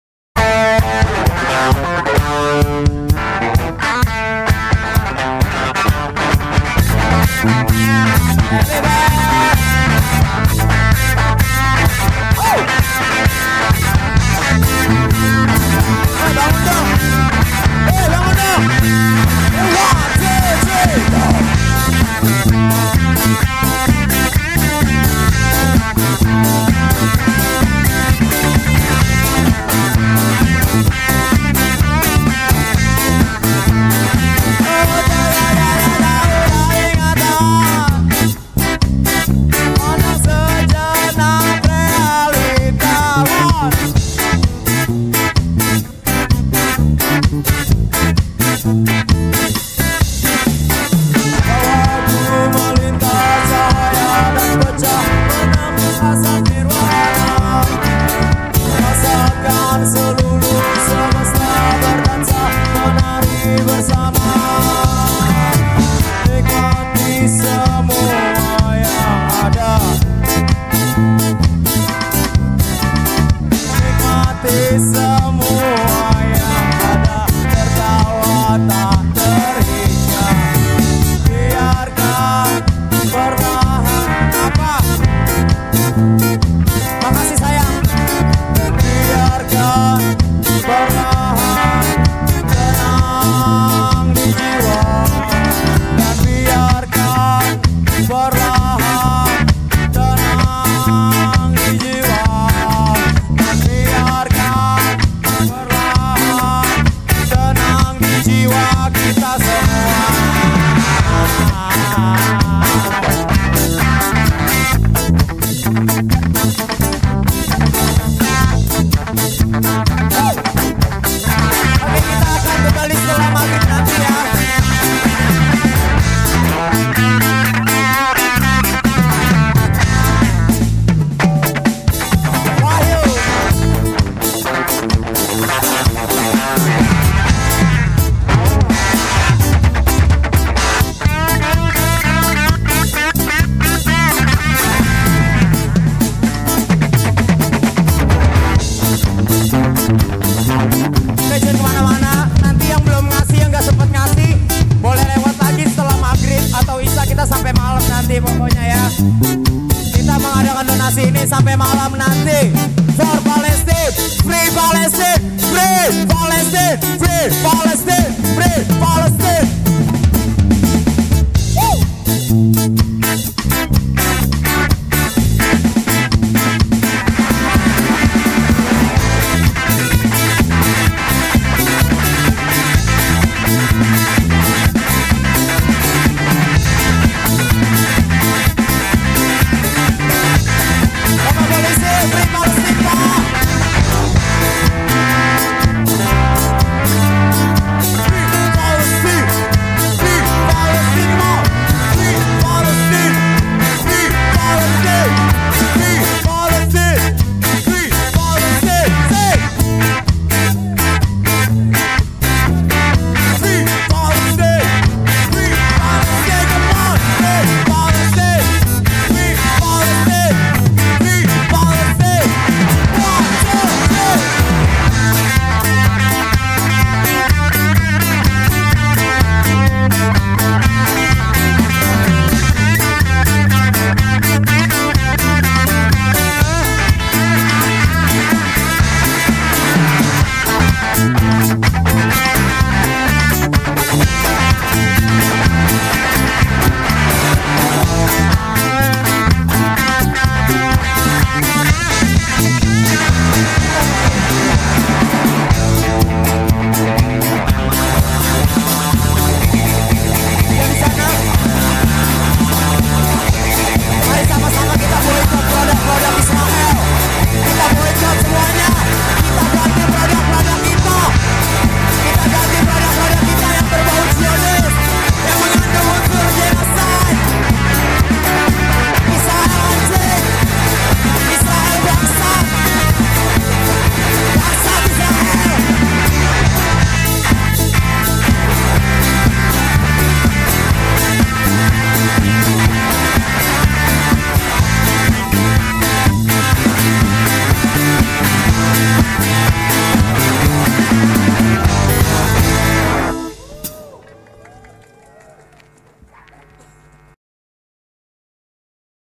Penggalangan Dana Untuk Palestina
Bekasi 19 November Tepatnya di depan Kecamatan Babelan Bekasi, kami pemuda pemudi menyenggarakan Musik amal yang dimana Hasil yang kami dapatkan dari kegiatan tersebut kita sumbangkan untuk Negara Palestina, semoga apa yang kami berikan bisa Bermanfaat

Salam kedamaian.